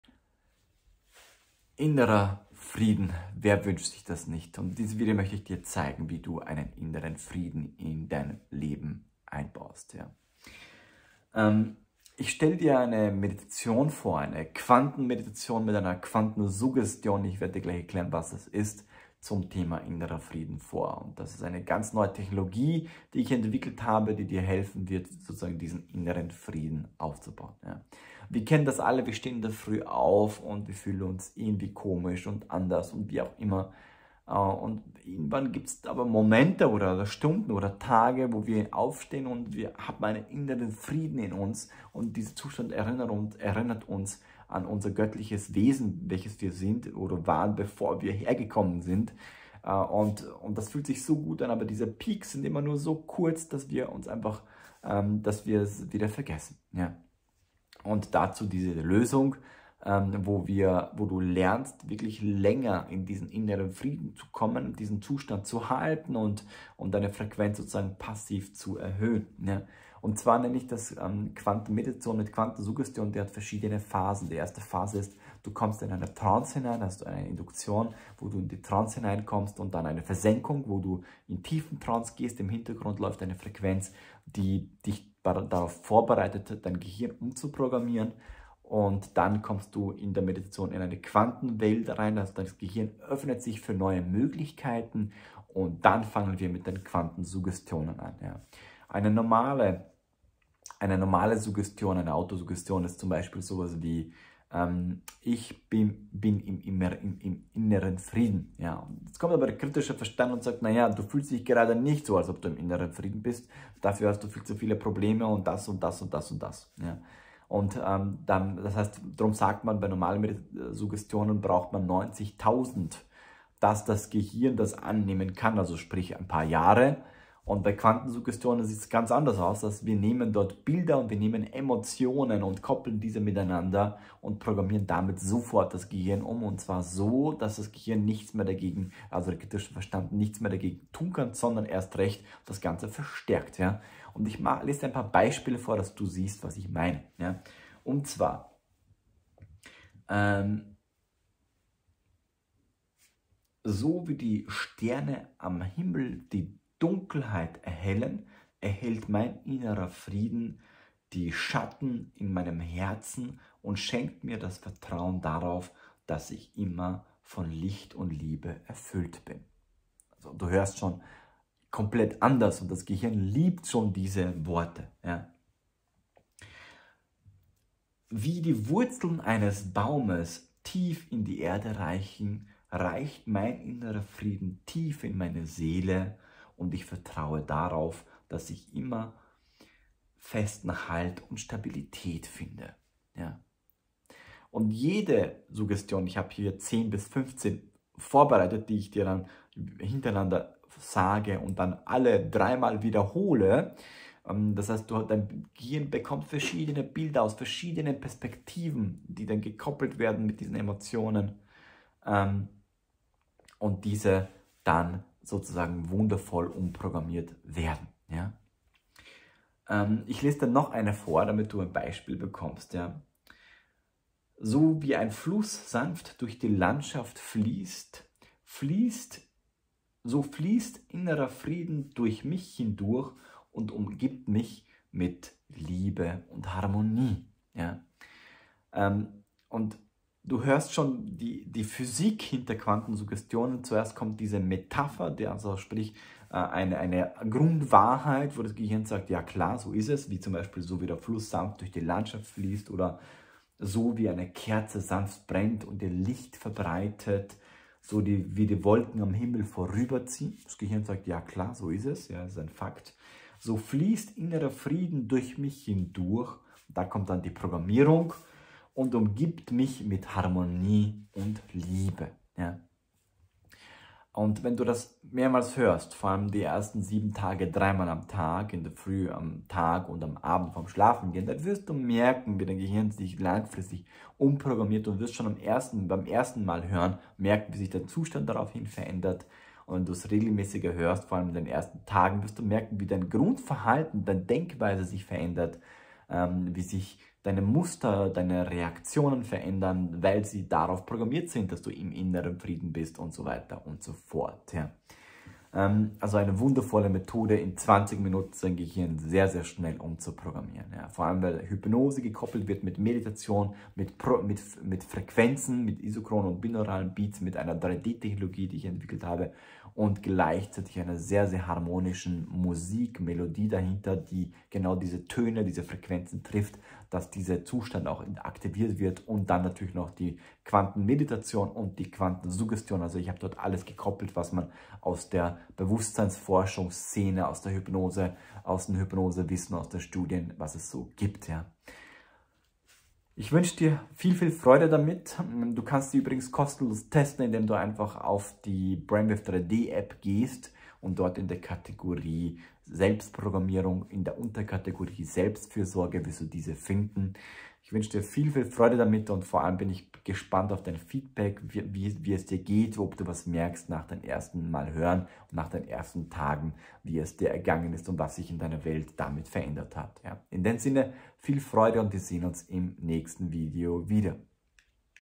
Meistere die Kunst des inneren Friedens: Finde innere Ruhe und Balance mit dieser Quantenmeditation
Entdecke die Quantum Inner Peace Meditation und meistere die Kunst von innerem Frieden und Balance. Lerne, wie du Stress abbauen, deine Gedanken beruhigen und eine tiefe innere Ruhe finden kannst. Begib dich auf eine kraftvolle Reise der Selbstentdeckung und transformiere dein Leben durch innere Harmonie.